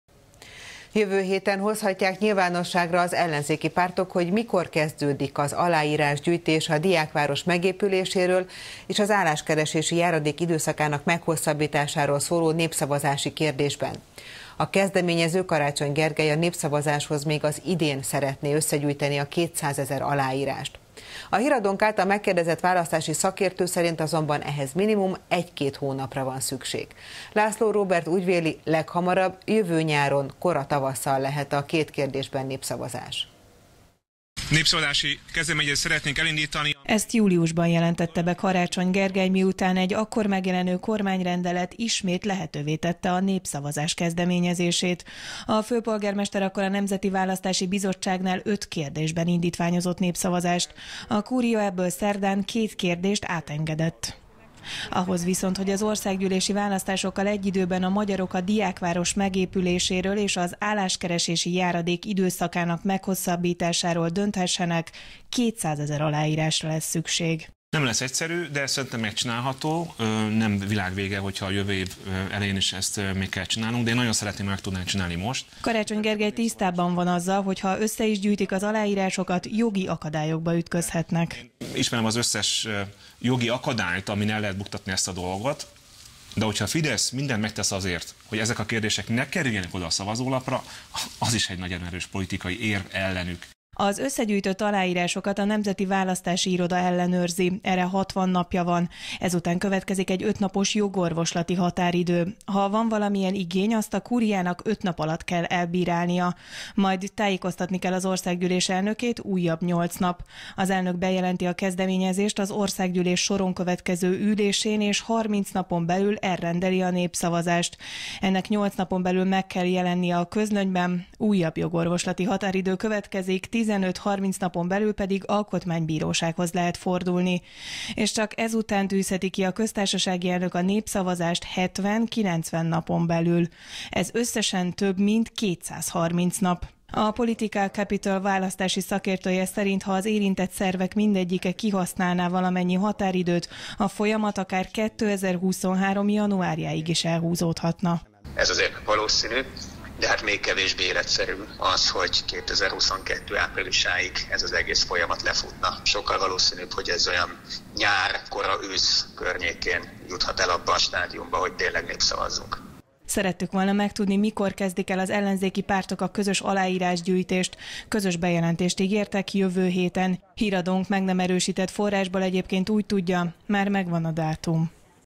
Szakértő: Nem reális, hogy Karácsony 200 ezer aláírást tervez gyűjteni idén a népszavazásra
Jövő héten hozhatják nyilvánosságra az ellenzéki pártok, hogy mikor kezdődik az aláírásgyűjtés a Diákváros megépüléséről és az álláskeresési járadék időszakának meghosszabbításáról szóló népszavazási kérdésben. A kezdeményező, Karácsony Gergely a népszavazáshoz még az idén szeretné összegyűjteni a 200 ezer aláírást. A Híradónk által megkérdezett választási szakértő szerint azonban ehhez minimum 1-2 hónapra van szükség. László Róbert úgy véli leghamarabb jövő nyáron, kora tavasszal lehet a két kérdésben népszavazás.